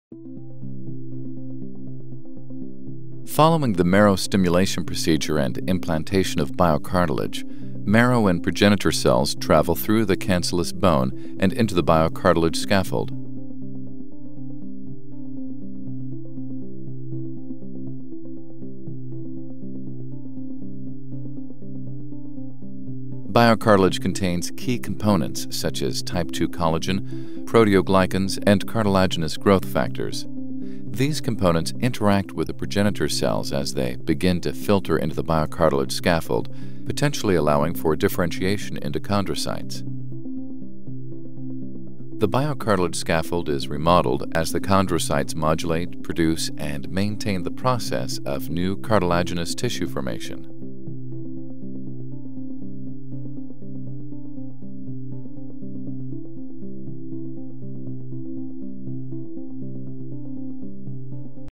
BioCartilage® Cartilage Extracellular Matrix
BioCartilage® provides a reproducible, simple and inexpensive method to augment marrow stimulation procedures. BioCartilage is developed from allograft cartilage and contains the extracellular matrix that is native to articular cartilage. This animation shows how BioCartilage may interact and potentially signal autologous cellular interactions to improve the degree and quality of tissue healing within a properly prepared articular cartilage defect.